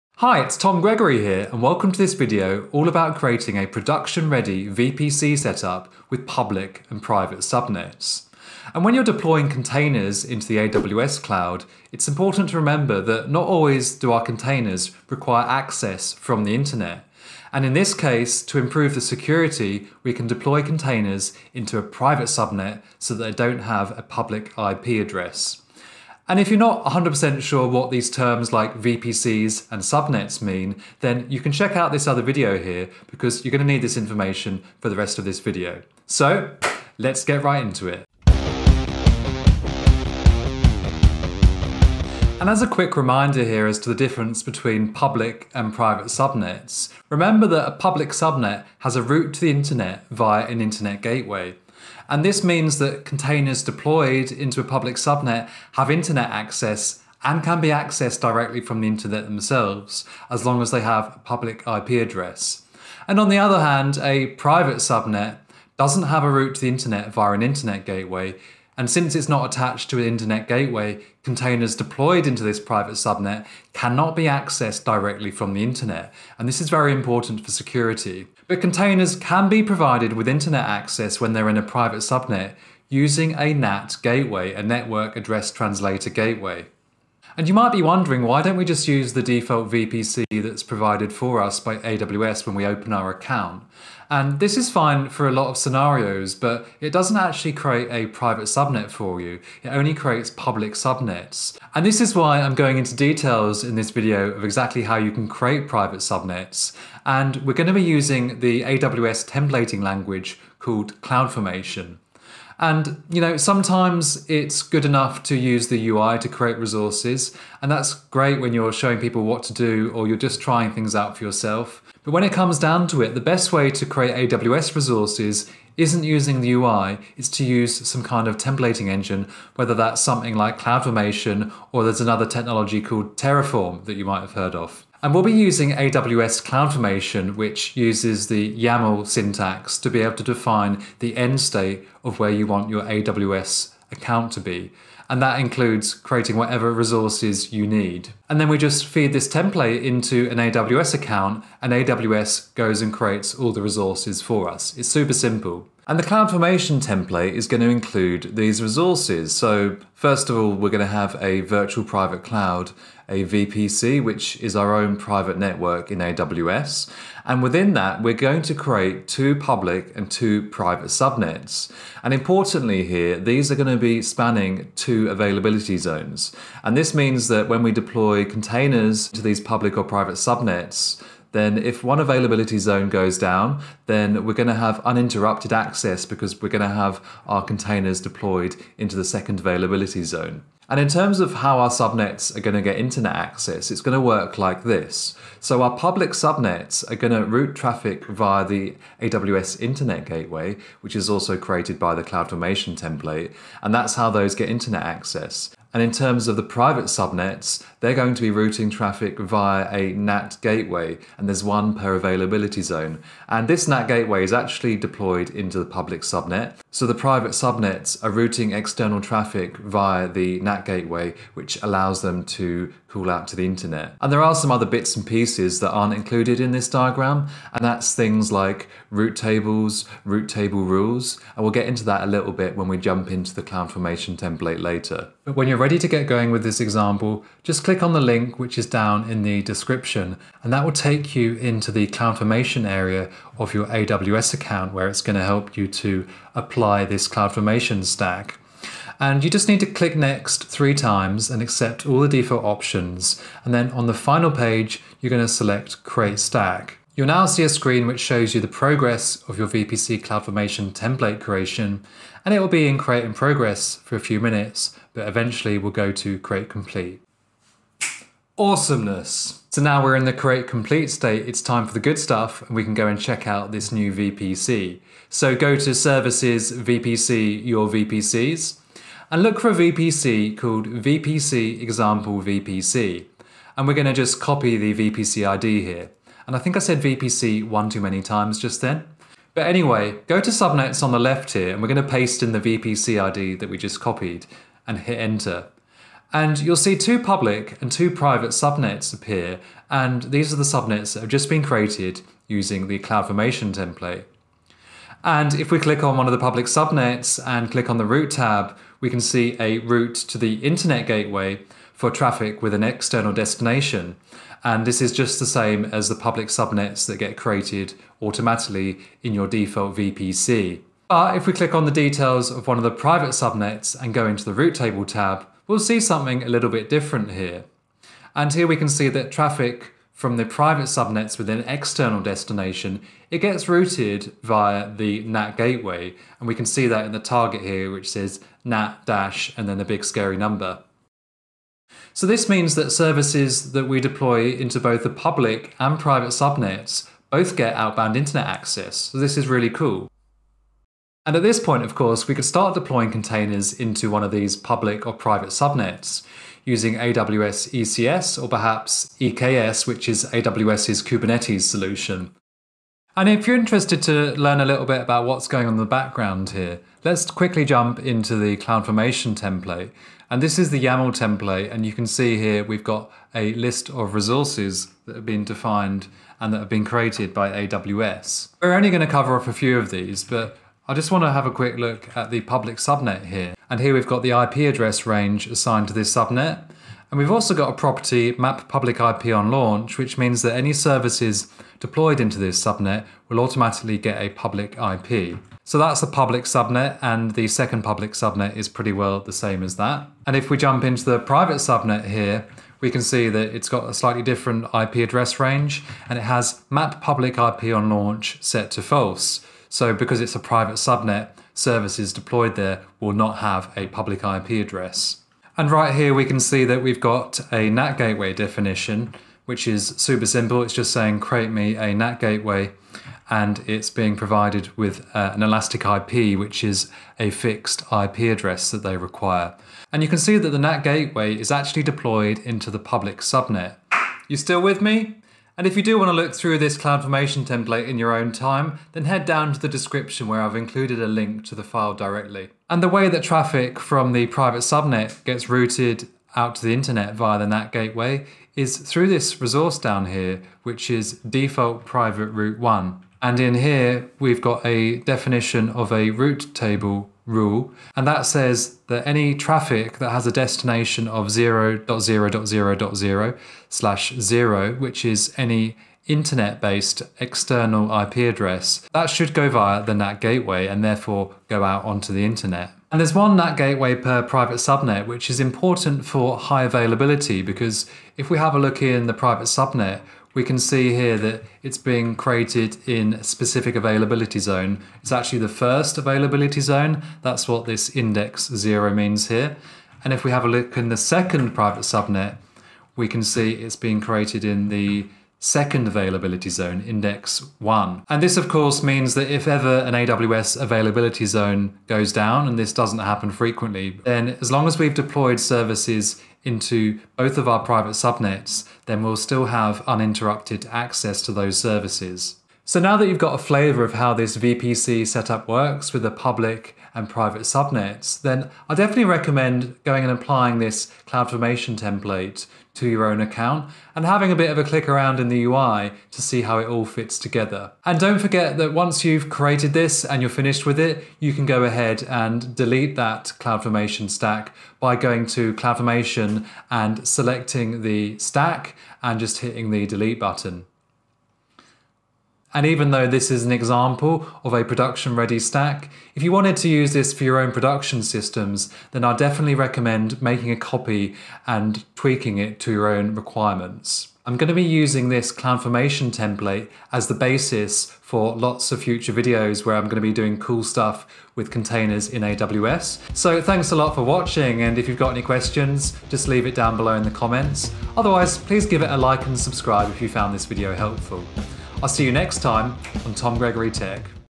Create public and private subnets in AWS VPC to secure your containers
Deploying containers into a VPC becomes more secure by creating them in a private subnet. This means they can't automatically be accessed from the internet. 🔒

Since you don't get private subnets in the default VPC in your AWS account, in this video I'll show you exactly how to create a VPC with public and private subnets. We'll be doing this using the templating language AWS CloudFormation, which means you can try out the example yourself.

▶️ Public vs. private subnets 0:42
▶️ CloudFormation recap 1:46
▶️ What AWS resources we're going to create 2:35
▶️ Applying the template to create the resources 3:55
▶️ Checking out our new VPC 4:35
▶️ Jumping into the CloudFormation template 6:02

✅ Here's the LAUNCH CLOUDFORMATION STACK link to create this VPC in your own AWS account

✅ Here you can view the CloudFormation template directly to see what AWS resources get created

✅ If you want to read the article that goes with this video, it's